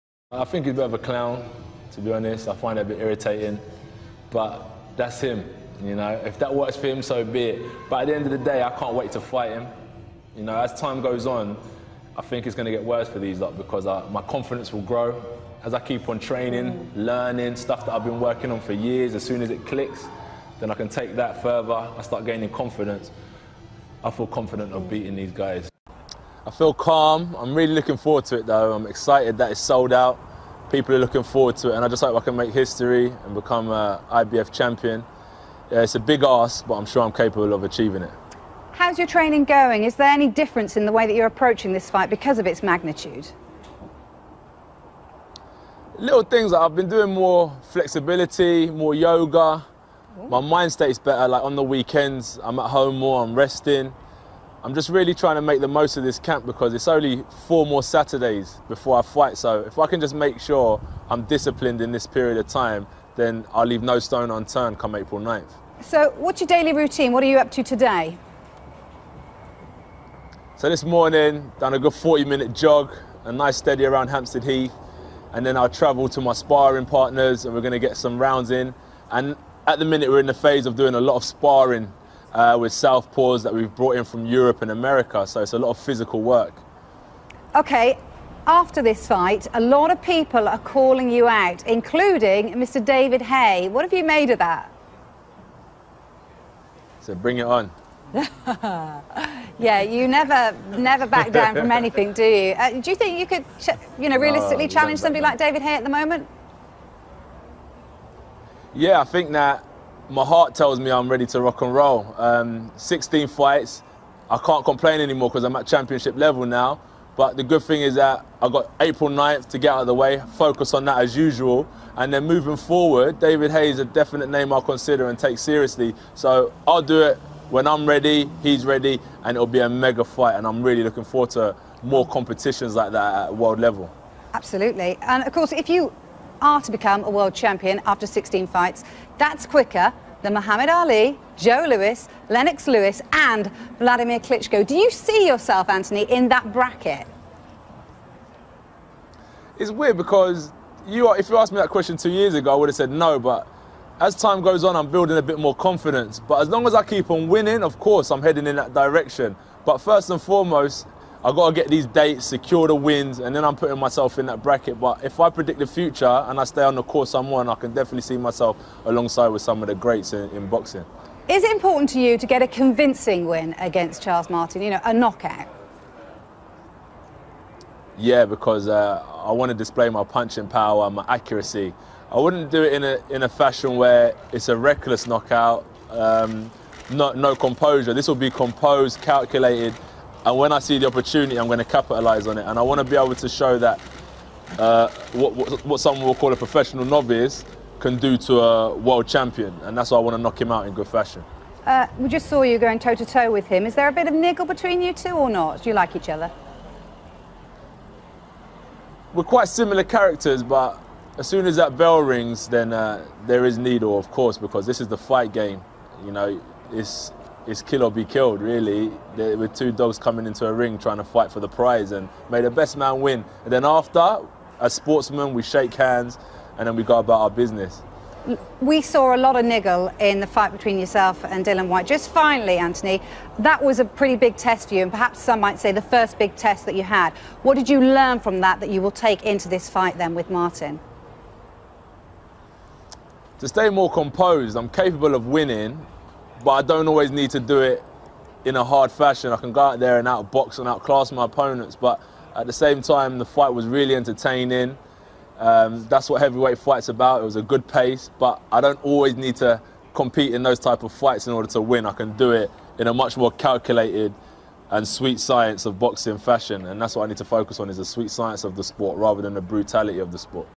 JOSHUA FIRES SHOTS AT TYSON FURY 'HIS A CLOWN' AND DAVID HAYE TO 'BRING IT ON'
Your support (ratings/comments/etc) is appreciated!

InstaSlam Your support (ratings/comments/etc) is appreciated!